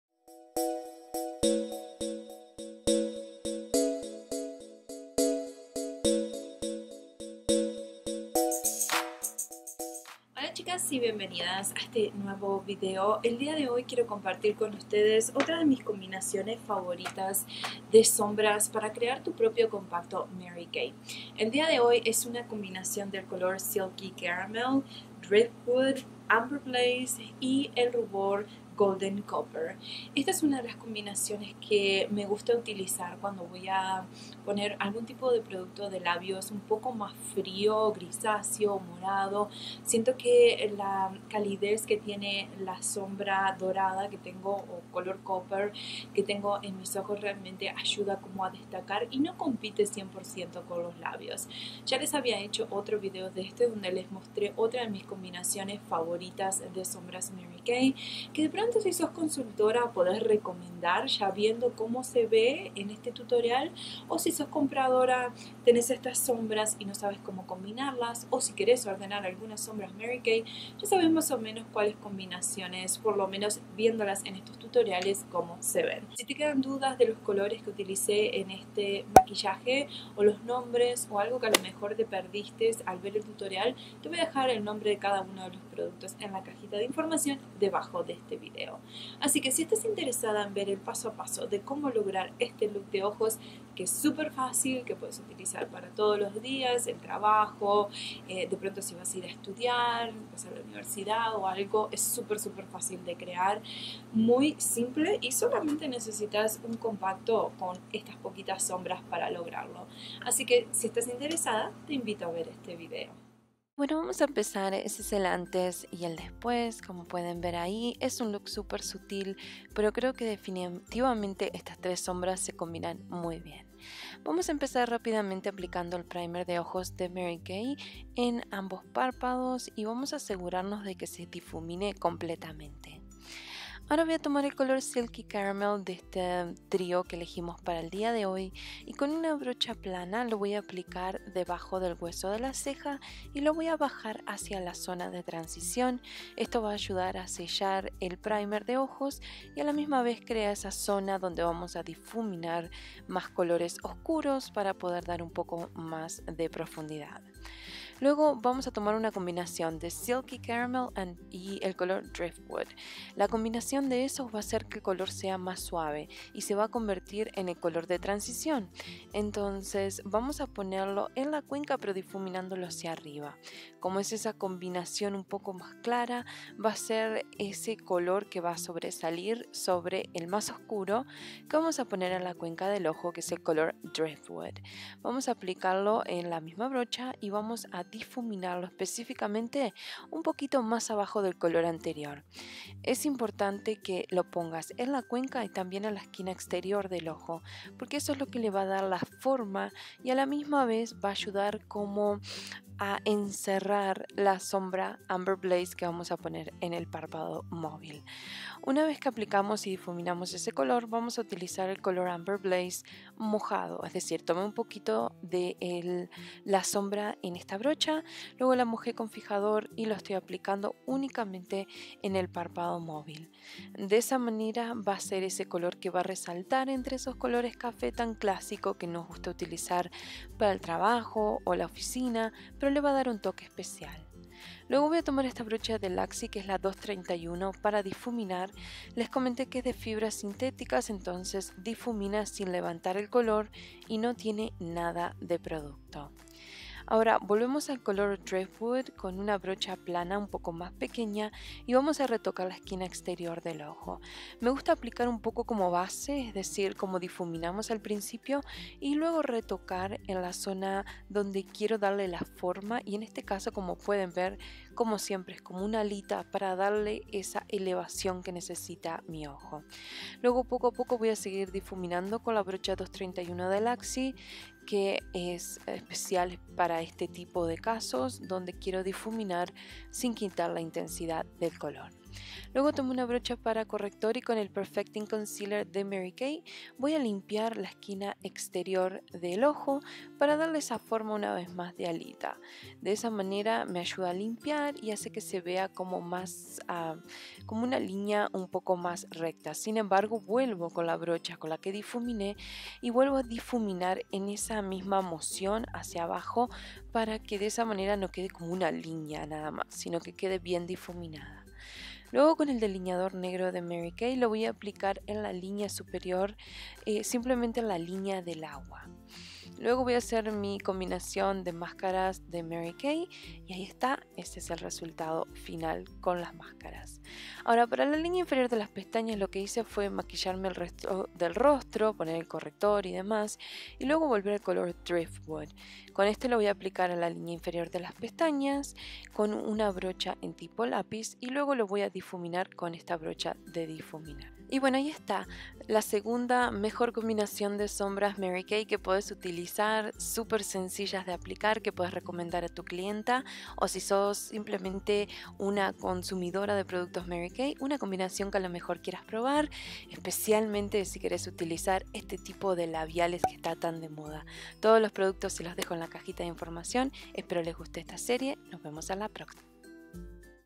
Crea tu Estuche Cosmetico Mary Kay| Las Mejores Combinaciones de Sombras Mary Kay
Este es el segundo video de la series" Las mejores Combinaciones de Sombras Mary Kay". En este caso use los colores driftwood, Amber Blaze,Silky Caramel & el rubor Golden Copper. Y les enseño el paso a paso de como combinarlas para que puedas recrear este maquillaje en casa.

Mary Kay® Compact Mini (unfilled)
Mary Kay® Ultimate Mascara™ | Black

Thanks!

Leticia Sanchez
Magna, UT, 84044.

♥Nos mantenemos en contacto por aquí !

✩S N A P C H A T | letisnaps

DONDE HAGO COMPRAS
✩Uso EBATES en todas mis compras y me devuelven cash